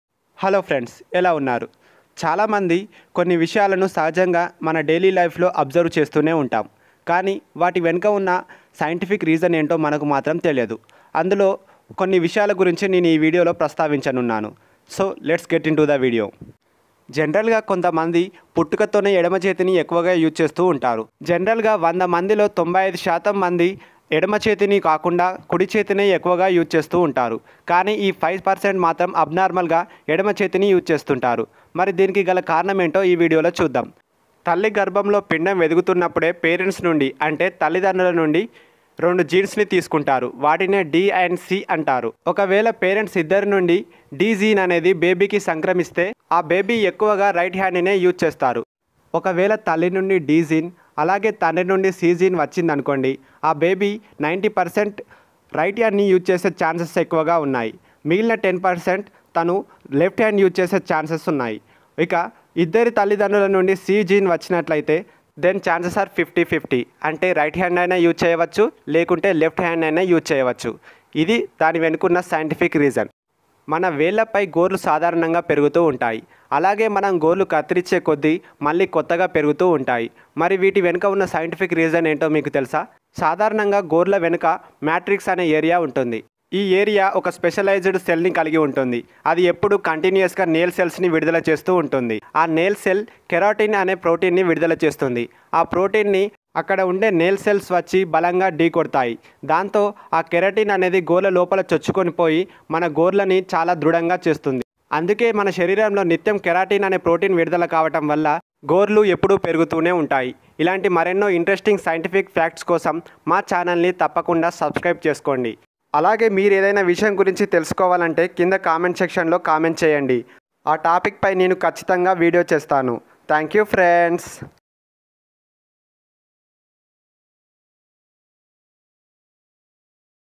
Why Are Some People Left-Handed? Scientific Reason Behind it | Interesting Facts in Telugu | MS NEWS
Hello Friends...today in this video I'm going to explain you the why are some people left handed? What will be the scientific reason behind it. And also how nails will grow? What are the interesting facts behind it?

Thanks!!